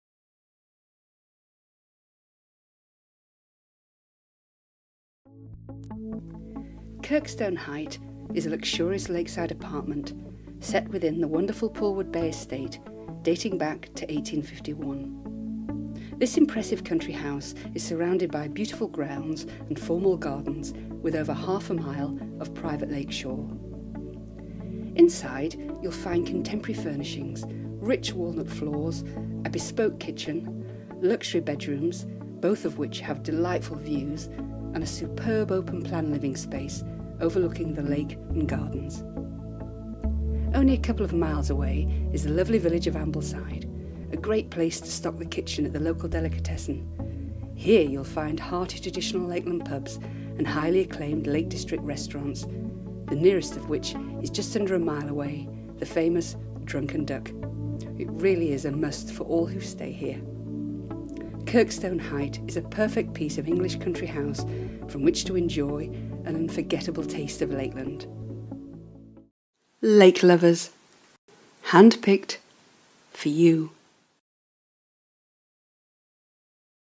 Kirkstone Height, Pullwood Bay
A luxurious lake shore apartment in perhaps the most prestigious setting in the Lake District. As part of the magnificent Pullwood Bay residence, Kirkstone Height provides a decadent retreat for one or two couples, family or friends on the serene shores of Lake Windermere, only 2 miles from Ambleside village. Within 50 acres of grounds and gardens, along over half a mile of private lake frontage, this imposing historic building has been converted with no expense spared.